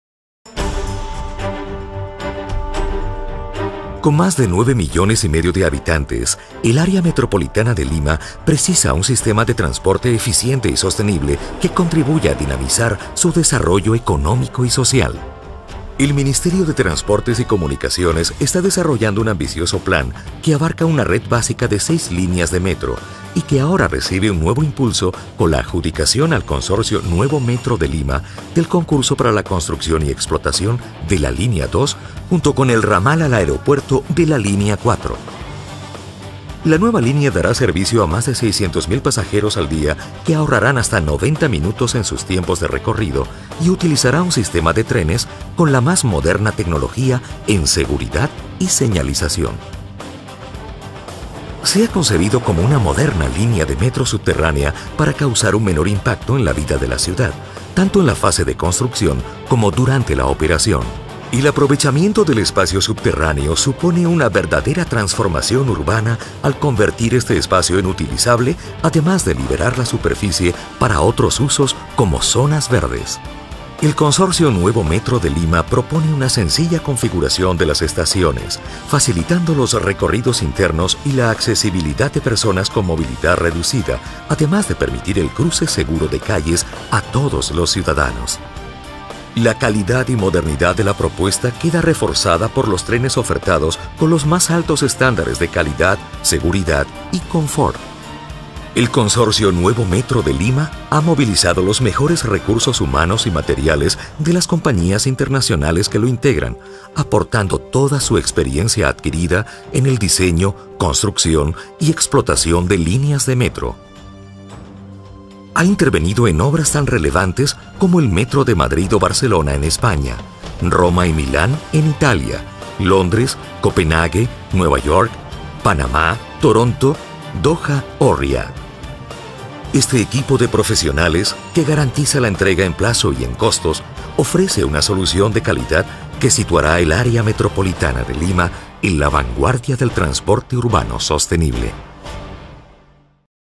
LIMA -PERU nuevo tren subterraneo 2014-2018.
SOMOS un pais humilde-primero el metropolitano de buses-el tren electrico, y ahora el nuevo tren subterraneo de lima. tendra un valor de 6500- millones de dolares